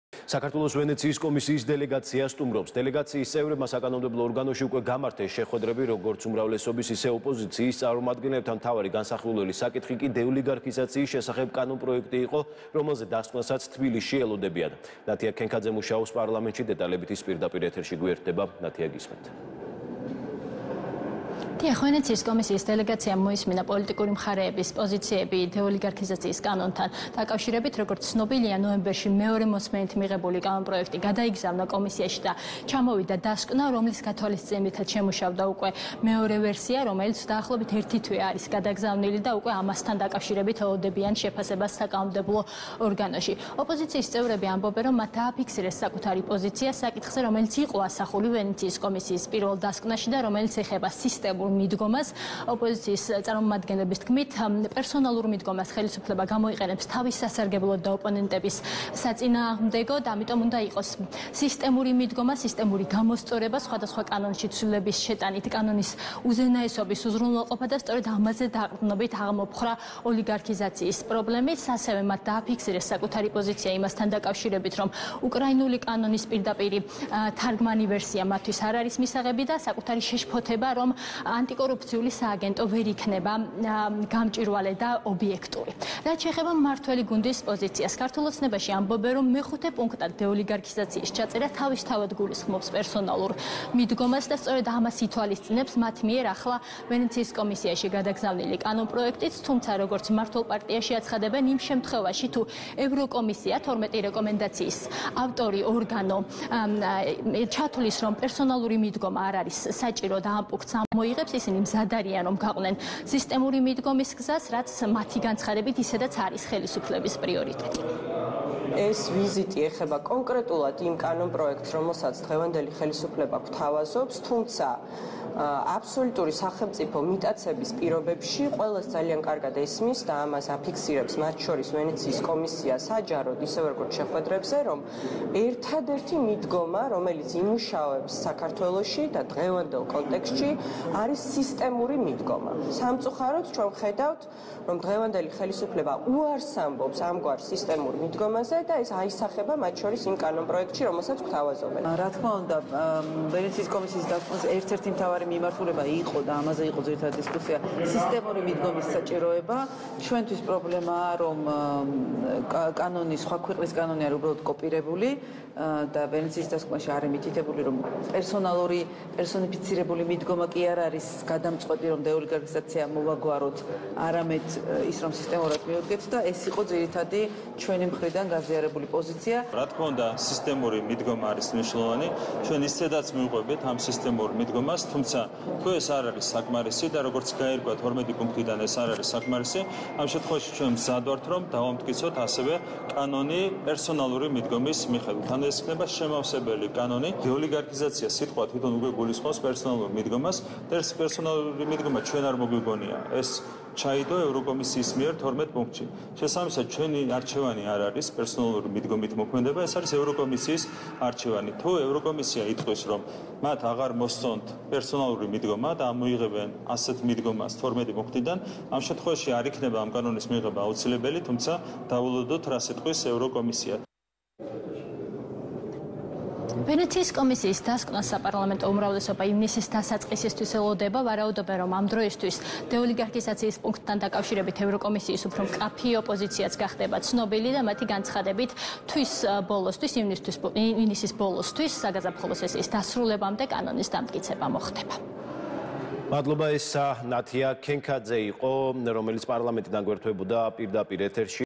ვენეციის კომისიის წარმომადგენლები თბილისში
საქართველოს ვენეციის კომისიის დელეგაცია სტუმრობს.
დელეგაციის წევრებმა საკანონმდებლო ორგანოში შეხვედრები უკვე გამართეს როგორც ოპოზიციის, ასევე უმრავლესობის წარმომადგენლებთან. მთავარი განსახილველი საკითხი დეოლიგარქიზაციის შესახებ კანონპროექტი იყო, რომელთან დაკავშირებითაც მხარეები კომისიის დასკვნას ელოდებიან.